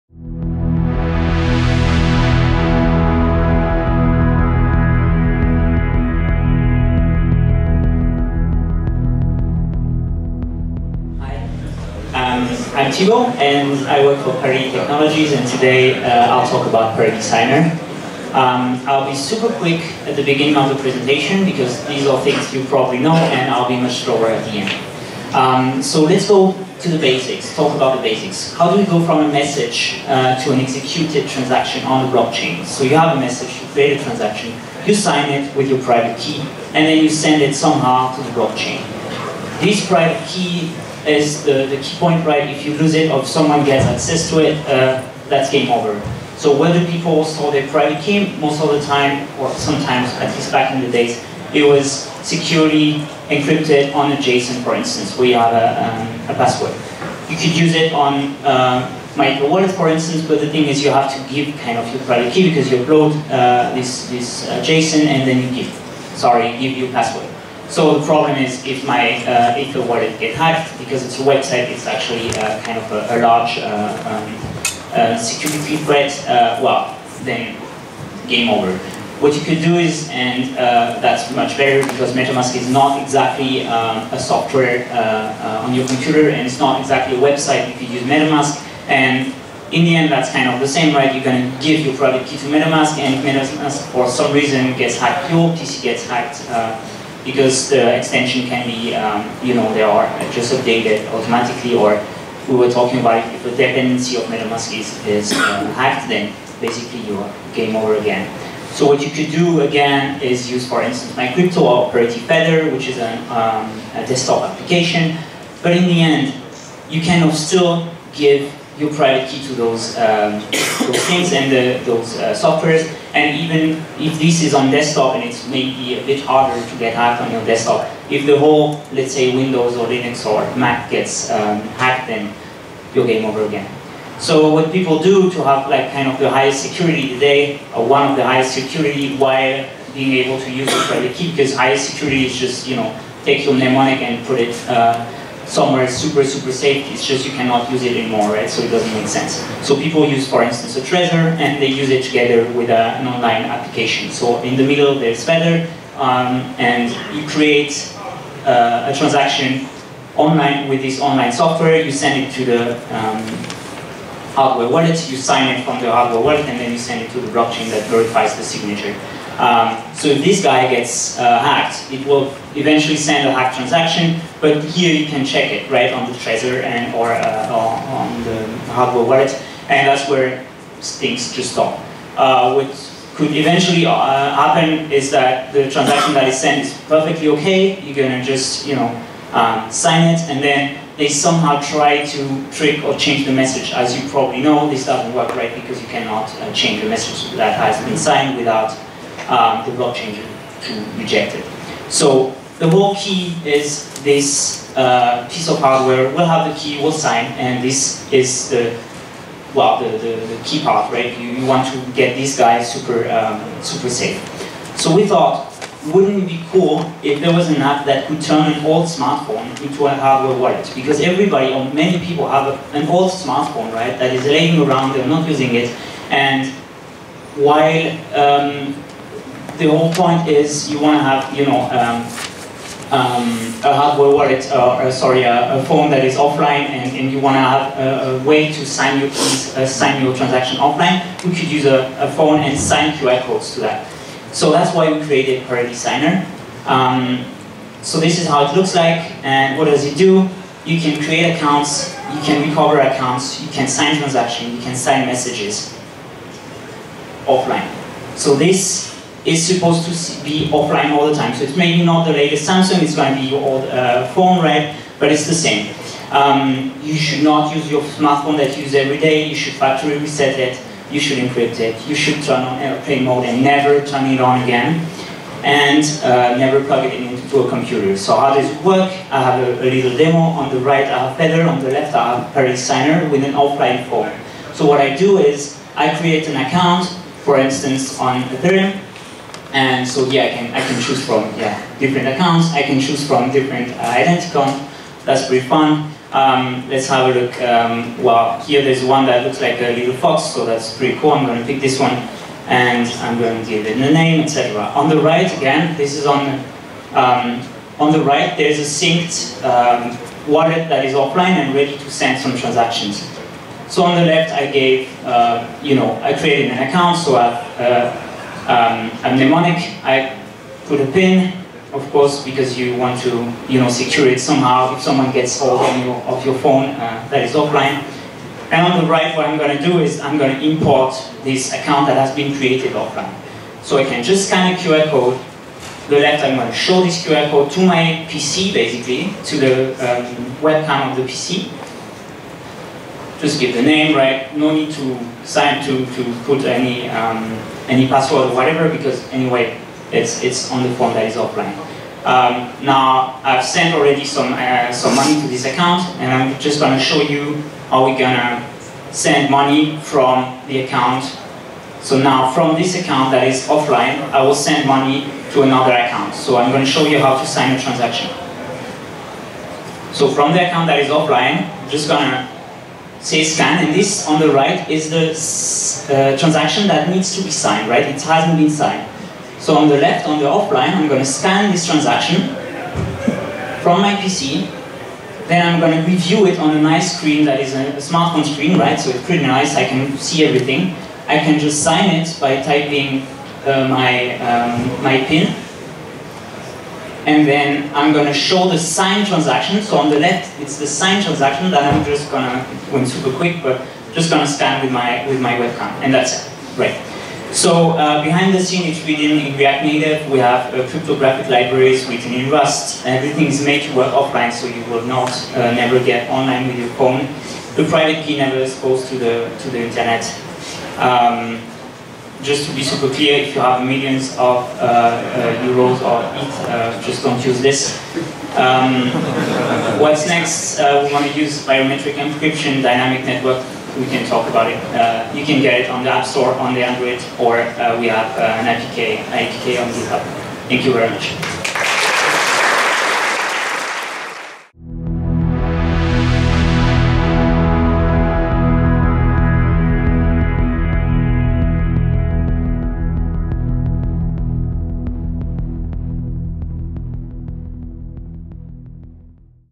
Parity Signer: Turn your smartphone into a hardware wallet by Thibaut Sardan (Devcon5)
The best way to keep your private key safe, is to keep it disconnected from the internet. This is usually done in hardware wallet, however most of those wallets are directly connected to a computer, either via usb or bluetooth. What if the wallet was never online and never ever connected to an online device. This is achievable using QR code. We did it with Parity Signer. I will present why we built it, how, do a short live demo, and what we will do in the future.

Speaker(s): Thibaut Sardan
Skill level: Beginner
Track: Security

Devcon 5 was held in Osaka, Japan on Oct 8 - 11, 2019.